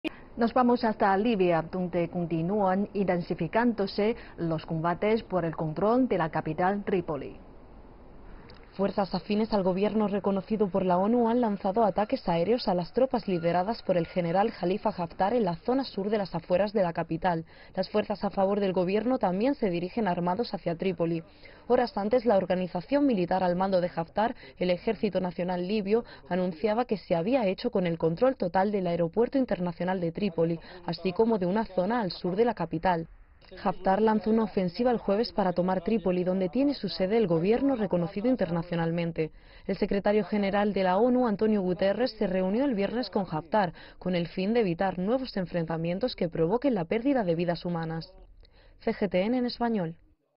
Batalla por Trípoli: Las fuerzas a favor del gobierno lanzan ataques aéreos a las tropas de Haftar
Continúan intensificándose los combates por el el control de la capital, de Libia. Fuerzas afines al gobierno reconocido por la ONU han lanzado ataques aéreos a las tropas lideradas por el general Jalifa Haftar en la zona sur de las afueras de la capital. Las fuerzas a favor del gobierno también se dirigen armados hacia Trípoli.